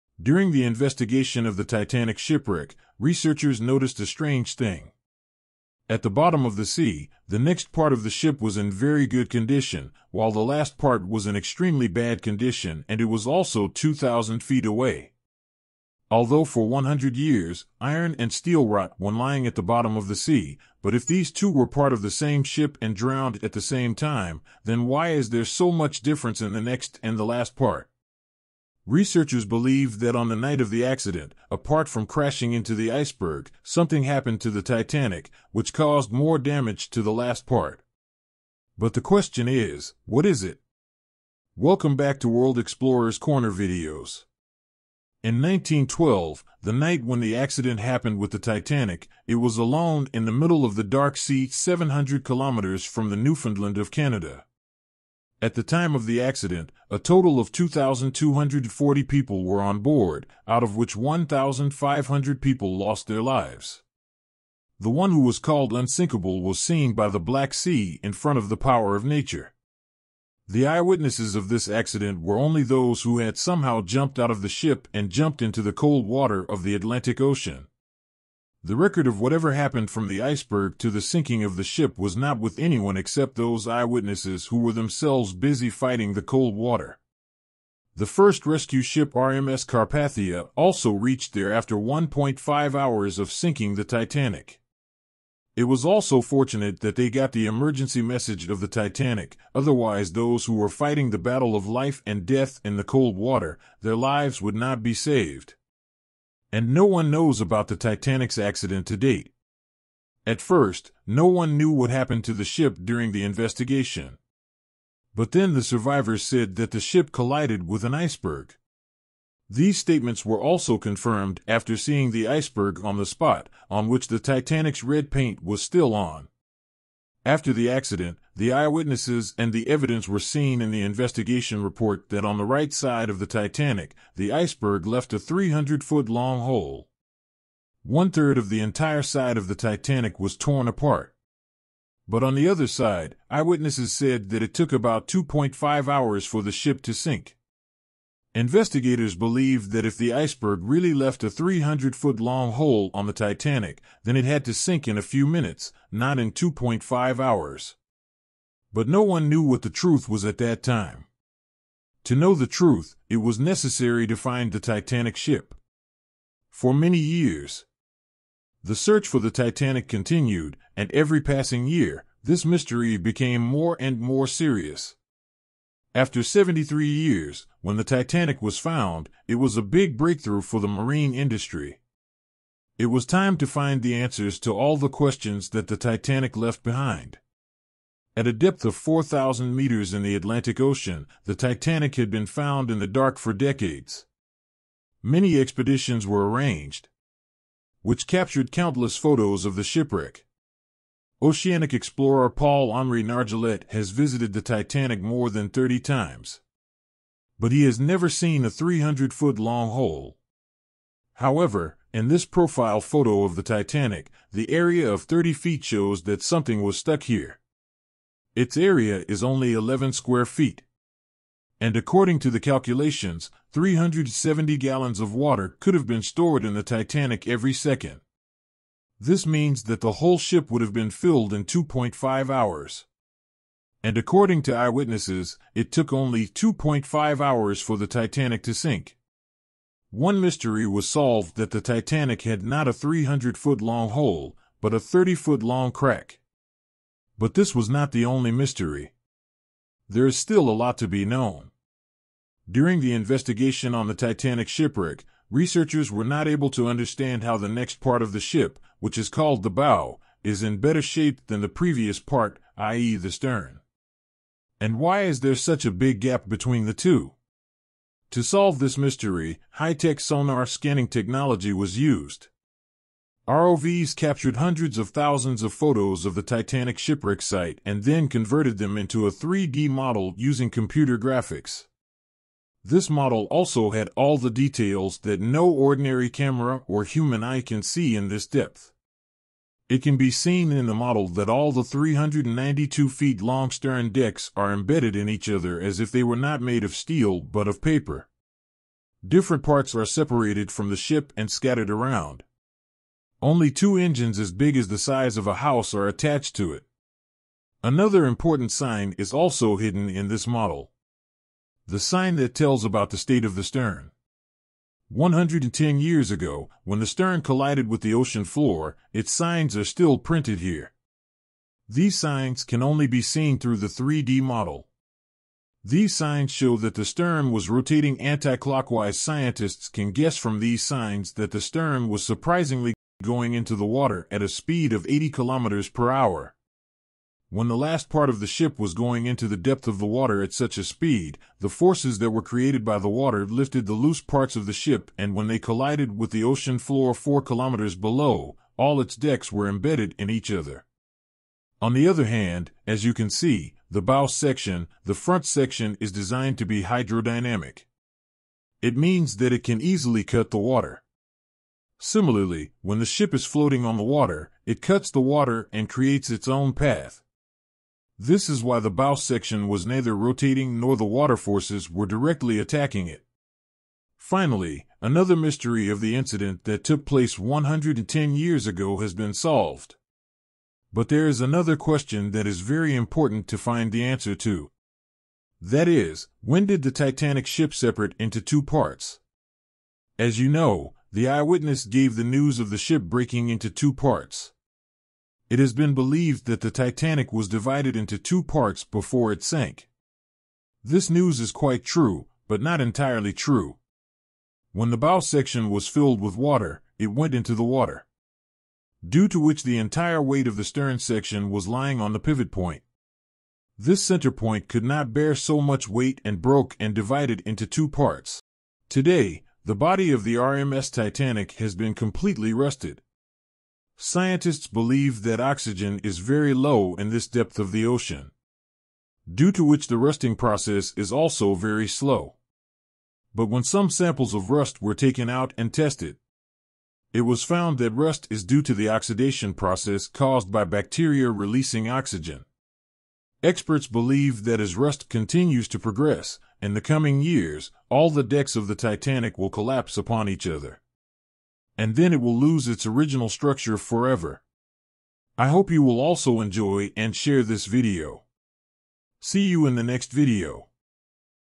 Mystery of Titanic | 110-Year-Old Mysteries Unveiled
Join us on an extraordinary journey to the depths of the Atlantic Ocean as we explore the long-lost wreckage of the RMS Titanic. In this captivating video, we reveal the astounding discoveries made by researchers during the investigation of the Titanic shipwreck. Witness the stunning 3D model created from high-tech sonar scans, which uncovers hidden details and sheds light on the intriguing differences between the ship's bow and stern sections.

Discover the truth behind the Titanic's collision with the iceberg and the surprising events that unfolded during its sinking. Delve into the mysteries surrounding the condition of the bow and stern sections, as we explain how they were affected differently during their descent to the ocean floor. Learn how the ship's stern rotation and water forces led to the peculiar arrangement of its decks, while the bow's hydrodynamic design helped it maintain a more intact structure.

Uncover the fascinating story of when and how the Titanic tragically split into two parts. Eyewitness accounts come to life as we examine the pivotal moment that caused the ship's stern to separate, forever changing the course of history. Additionally, we explore the rusting process that has slowly been altering the Titanic's wreckage over the past century and its potential implications for the future.

This riveting video brings you exclusive insights into the untold stories of the Titanic's final moments, supported by cutting-edge technology and expert analysis. Don't miss this captivating exploration of one of the most iconic maritime tragedies in history.